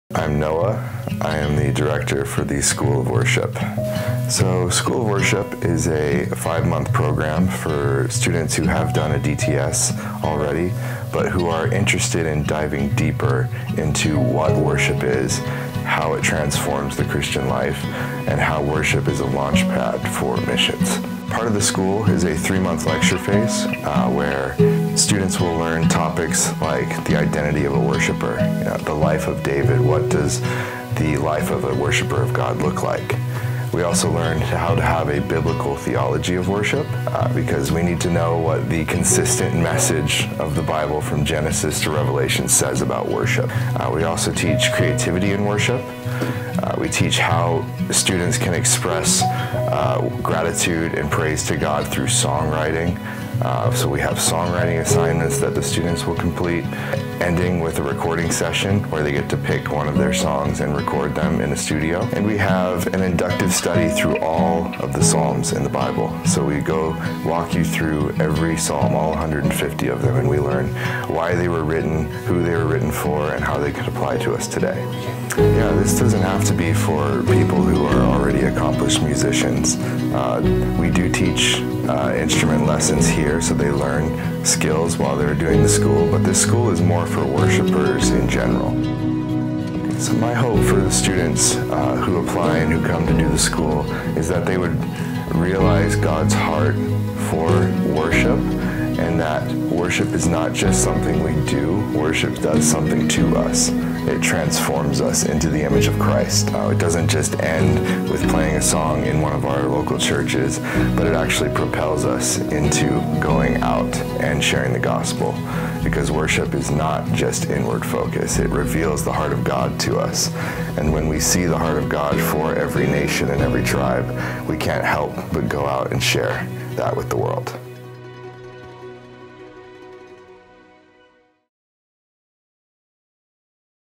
LEARN MORE ABOUT SCHOOL OF WORSHIP
SOW is a school that trains people who want to dive deeper into knowing what worship is, how it transforms us, and how worship is used as a launching pad into missions!